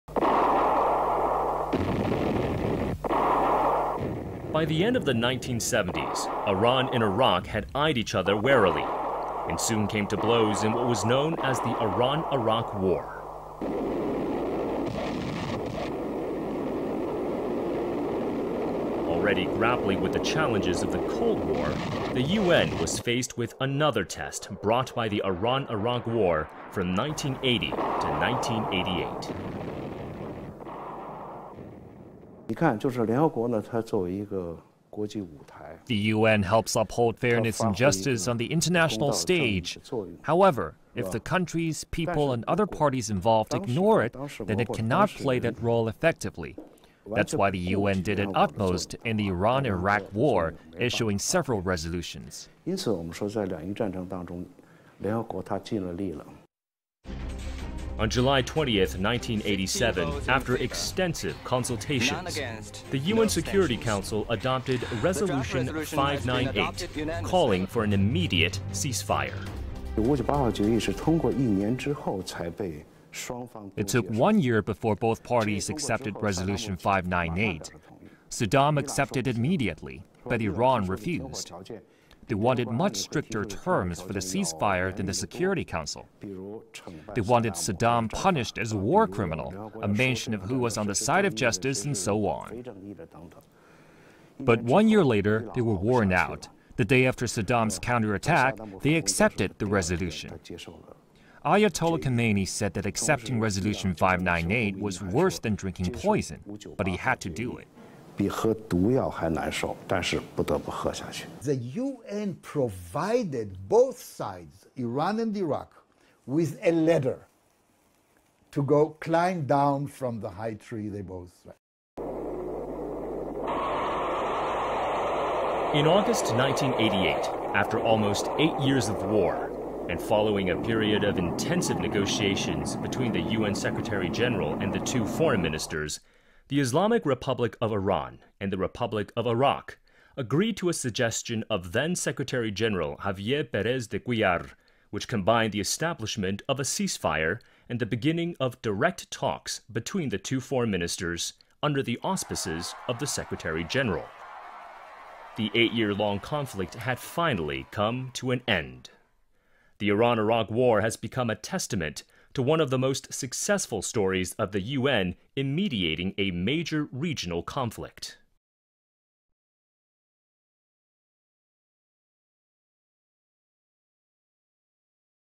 How did the Iran-Iraq war end under the auspices of UN mediation?
By the end of the 1970s, Iran and Iraq had eyed each other warily, and soon came to blows in what was known as the Iran-Iraq War. Already grappling with the challenges of the Cold War, the UN was faced with another test brought by the Iran-Iraq war from 1980 to 1988.